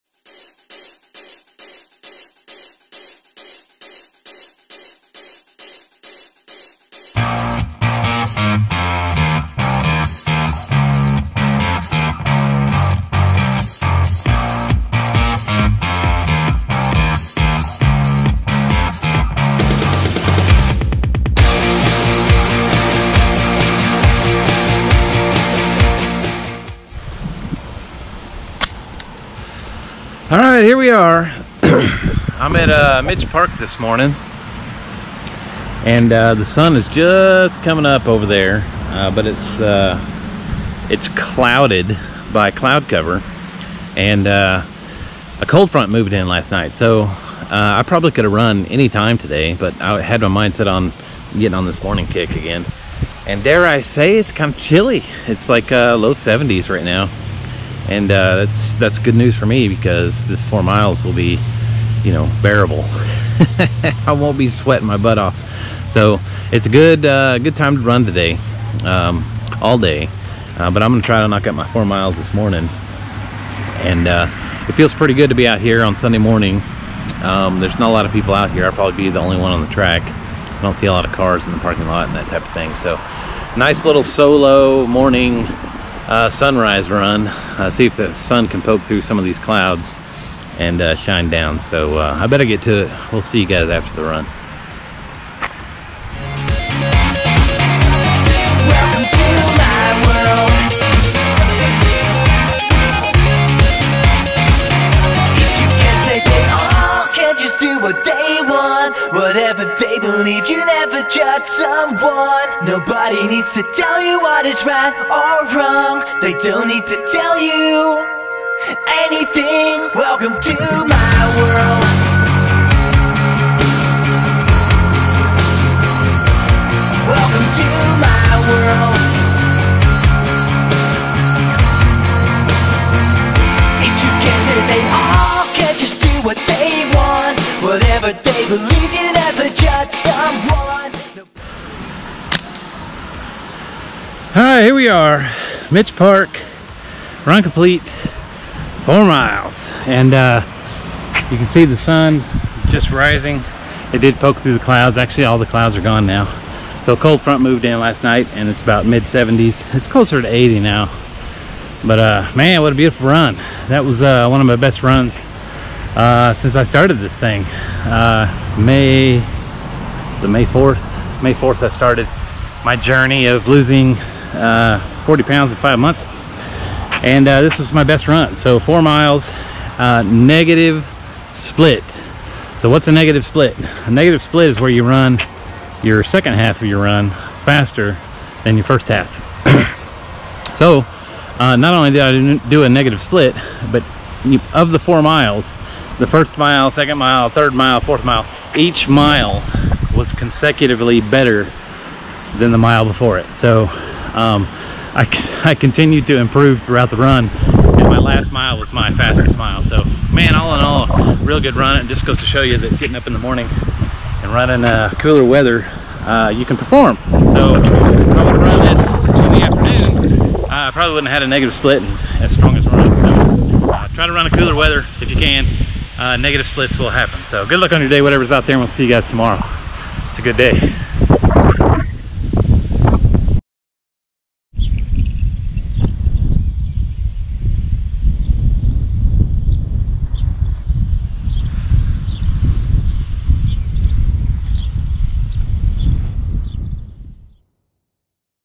NEGATIVE SPLIT RUN | DAY 22
NEGATIVE SPLIT RUN!  Well, a cold front moved in and this morning's running temperature was a cool 72 degrees and I took advantage of it!  I ran each mile of my 4 miler this morning faster than the mile previous.  I was full of energy and it just goes to show you that the weather plays an important role in your running performance!  I love this cold front!  Let's go!!

TOMORROW IS TOMORROW!